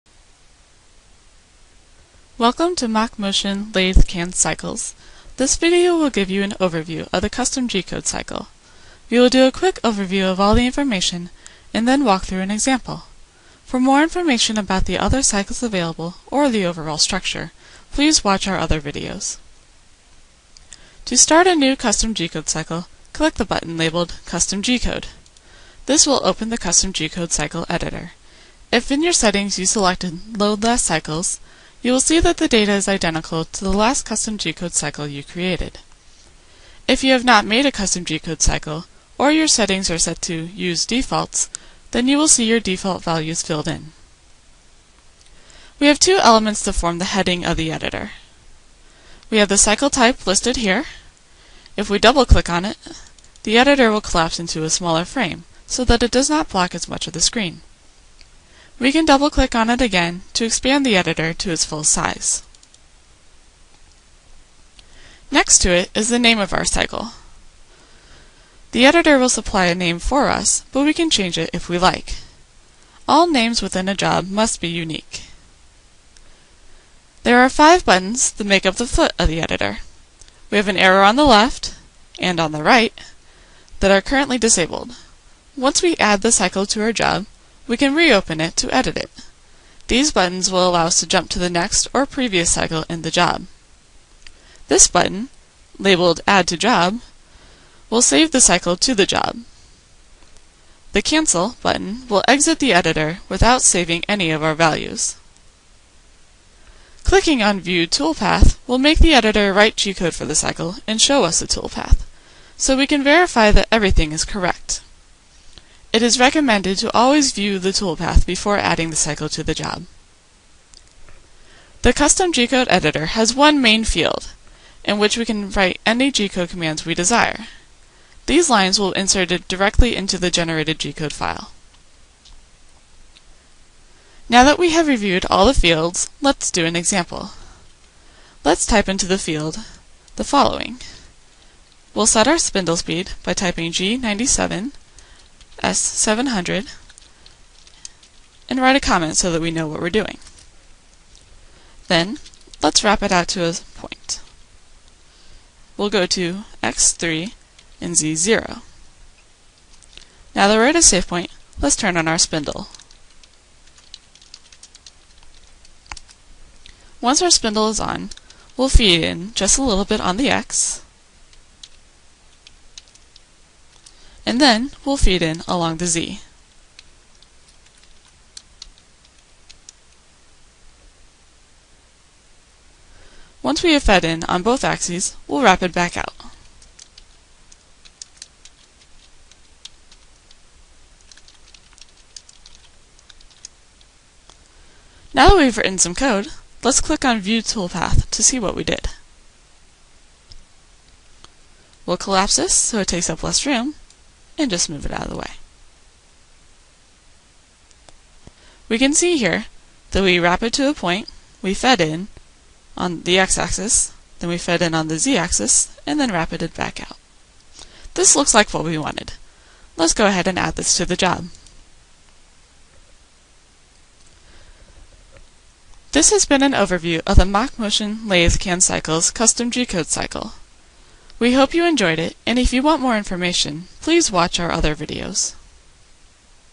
MachMotion Lathe Canned Cycles - Custom GCode Tutorial
Custom Gcode tutorial.

Watch an overview of the newly released MachMotion Lathe Canned Cycles, a conversational-type programming method for CNC lathe canned cycles such as turning, rounding, drilling, facing, grooving, taper and threading.

Available exclusively on MachMotion Lathe controls.